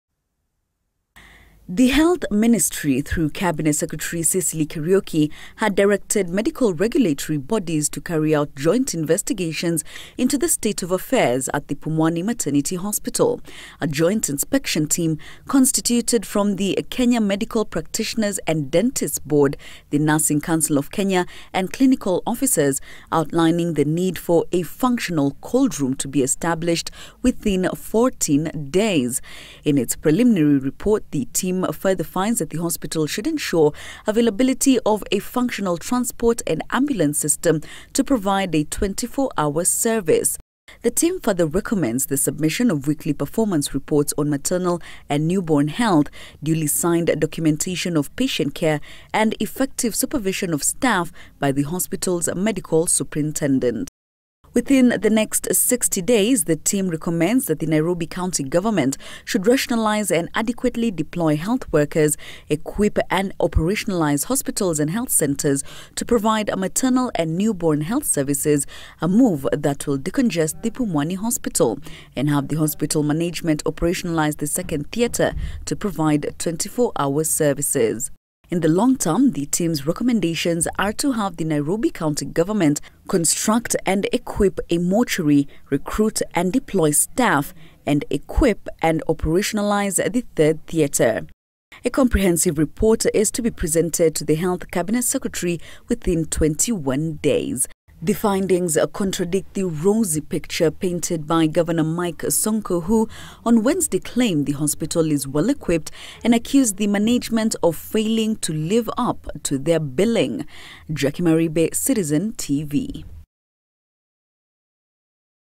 Probe team says Pumwani Hospital lacks equipment
A preliminary report on the state of Pumwani Hospital has uncovered the rot at the maternity facility, saying it lacks staff or equipment to serve mothers and their babies.